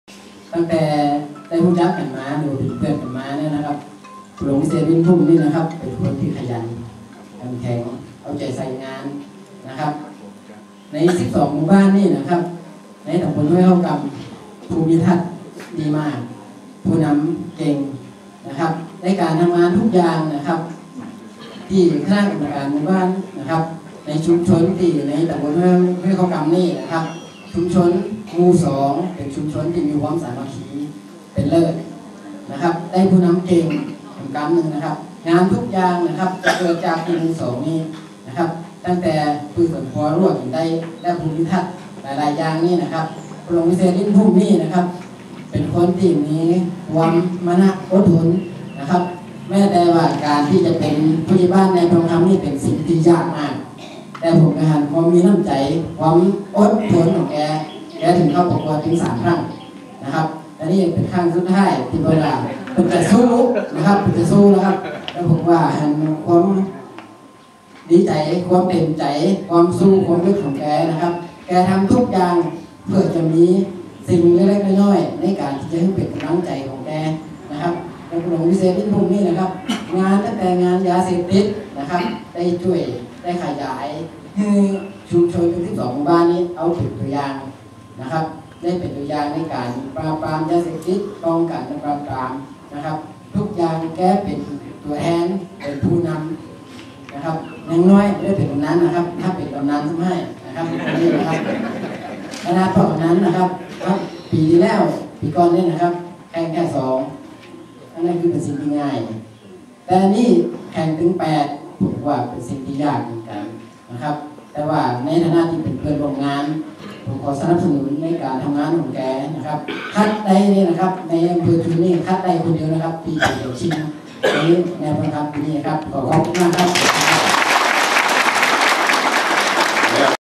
สนับสนุนผลงานนายวิเศษ รินทร์พรุ้ม
ตรวจ ประเมินผลงานผู้ใหญ่บ้านยอดเยี่ยม อ.จุน จ.พะเยา นายวิศษ รินทร์พรุ้ม ผญบ.บ้านทุ่ง ม.2 ต.ห้วยข้่าวก่ำ อ.จุน นำโดยนายสรธร สันทัด ปลัดจังหวัพะเยาและคณกรรมการ ได้รับการต้อนรับอย่างอบอุ่น (5กพ.2558)